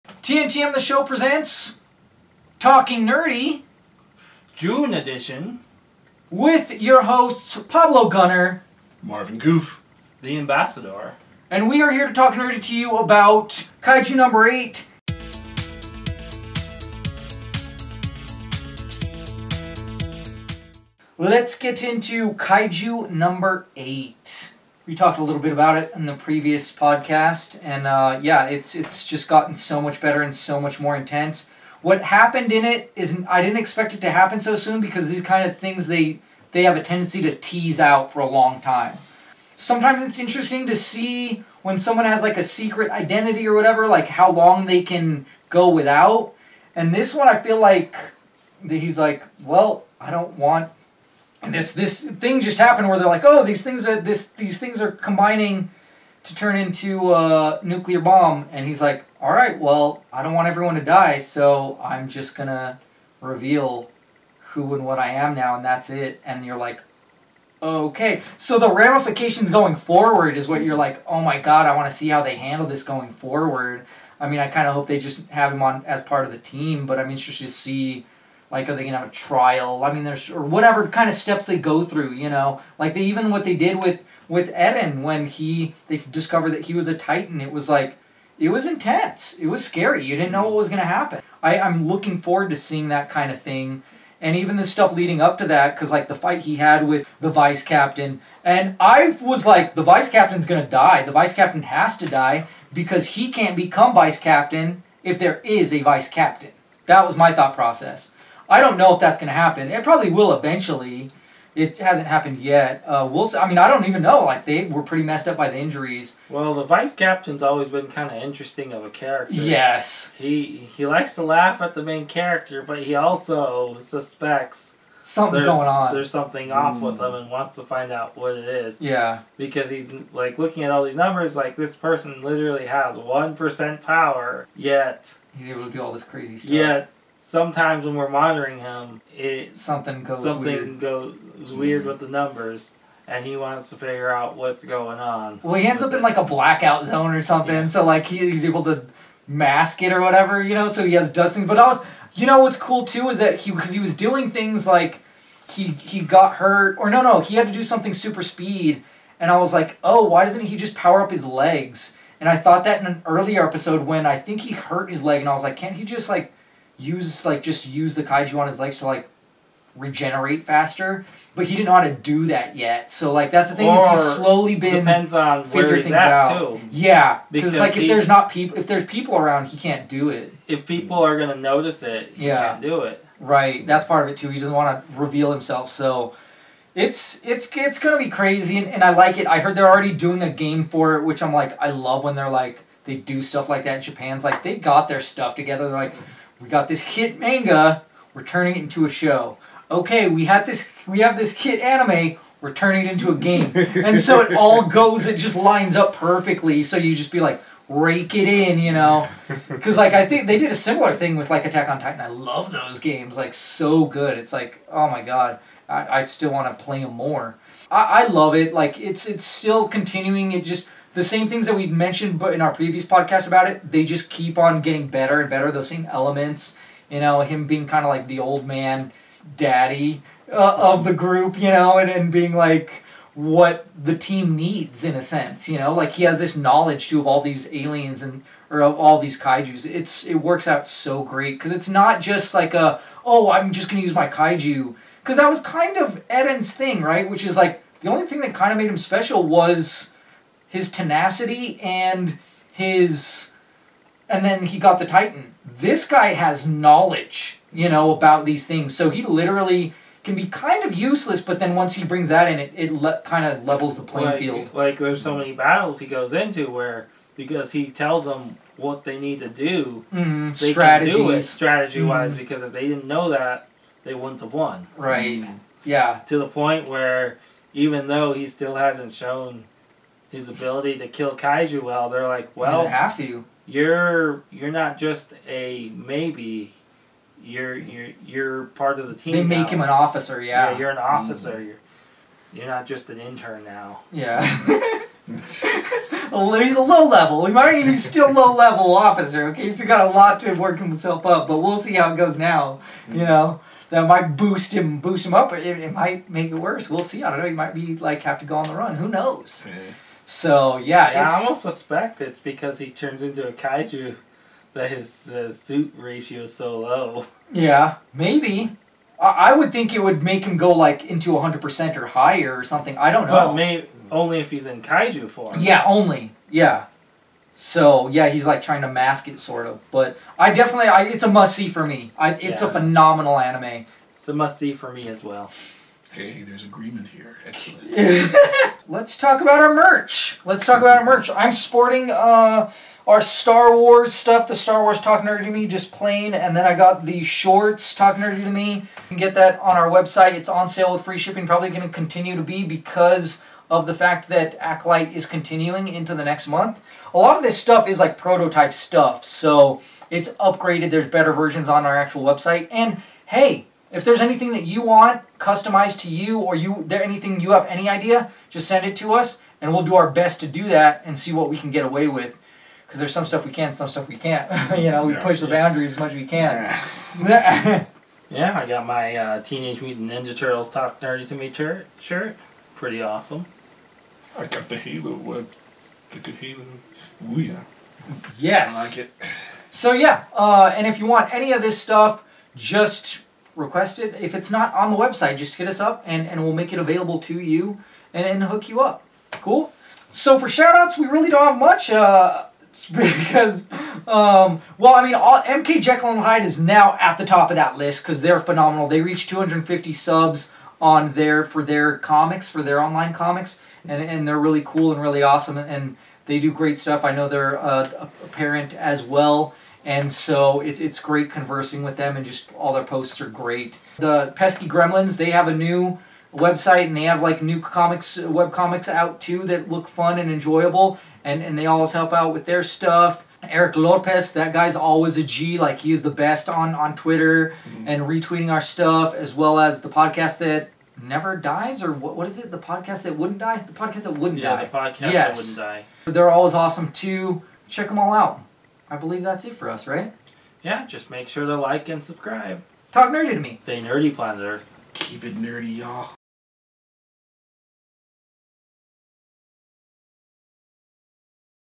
KAIJU No. 8 Review
Pablo Gunner, The Ambassador and Marvin Goof review Kaiju No 8